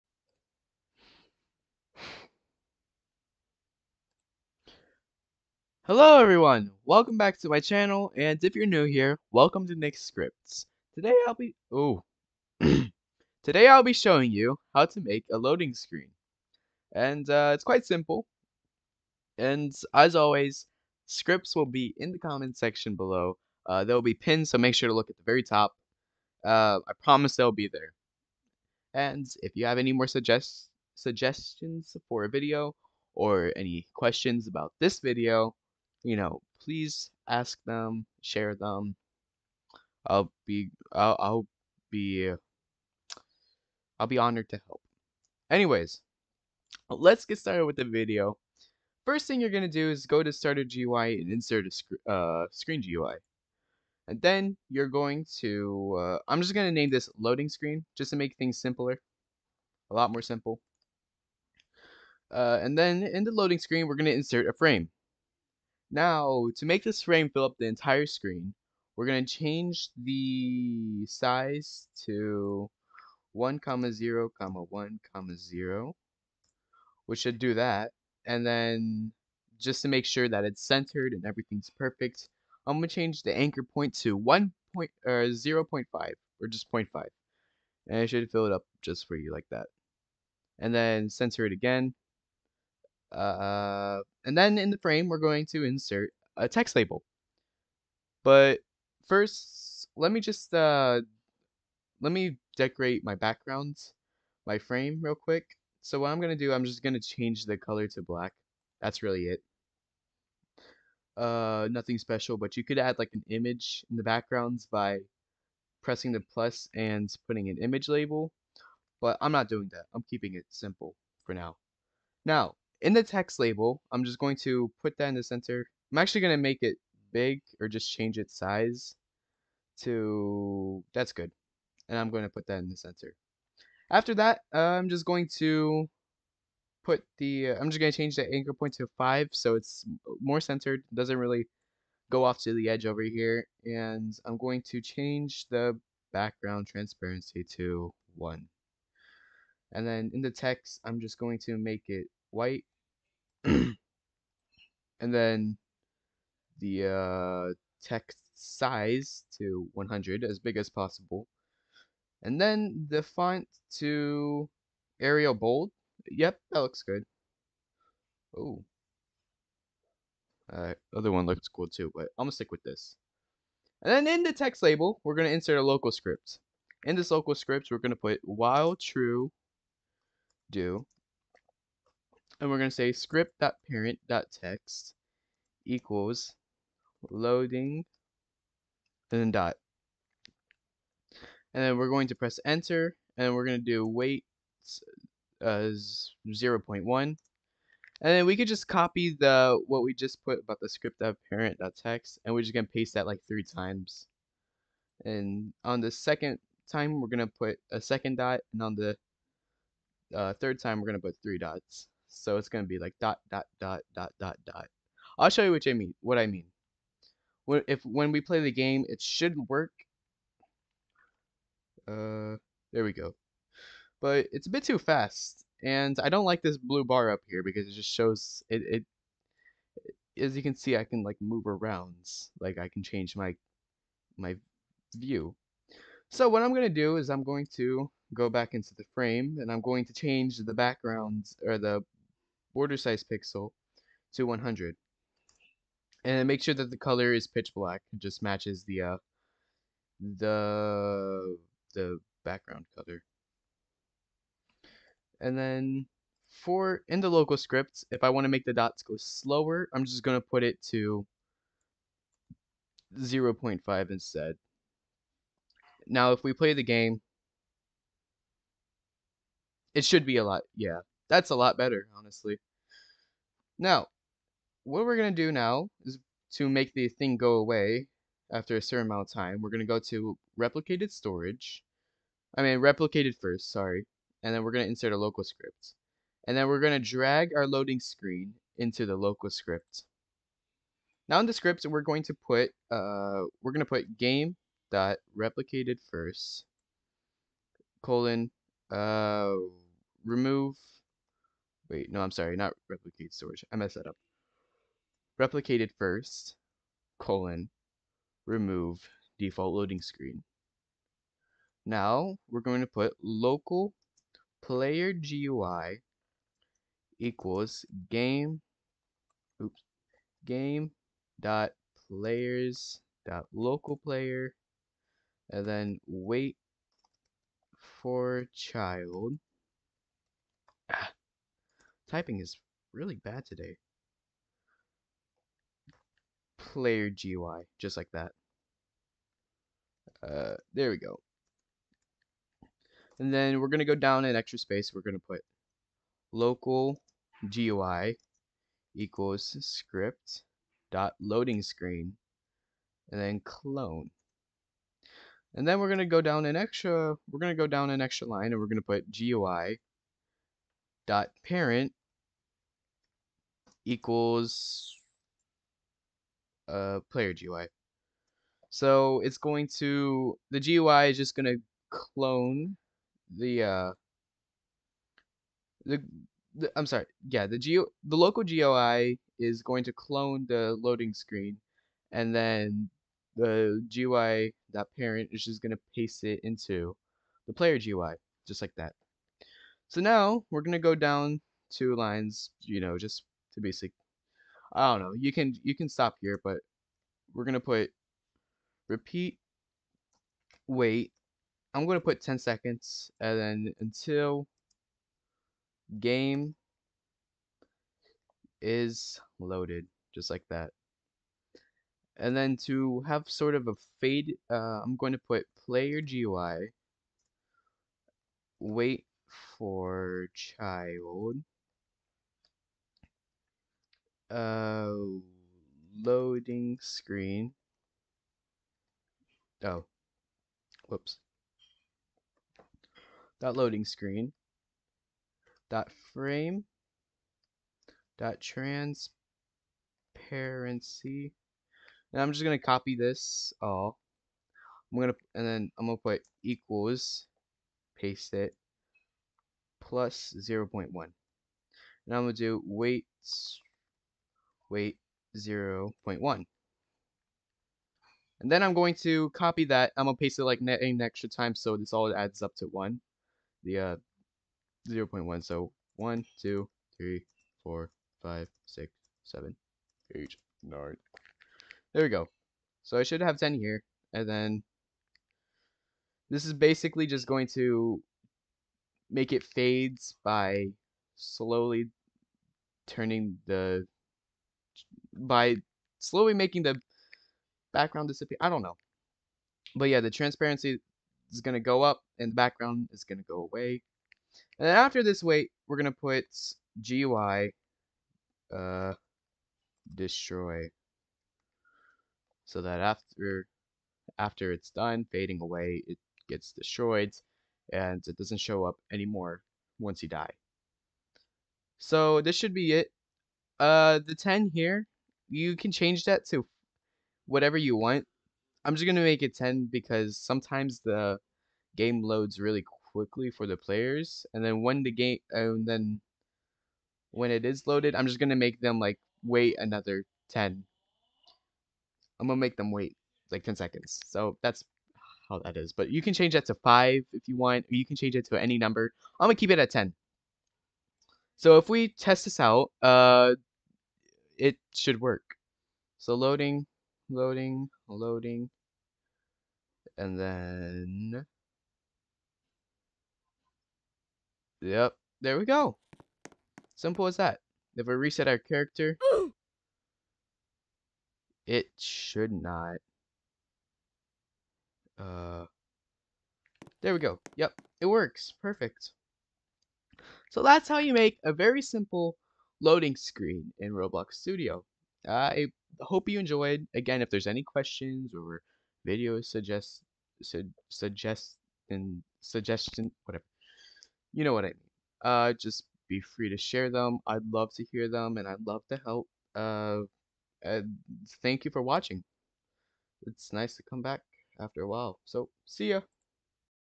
How to make a loading screen in Roblox Studio 2022
Sorry for the long break I lol. It was like a year and a half.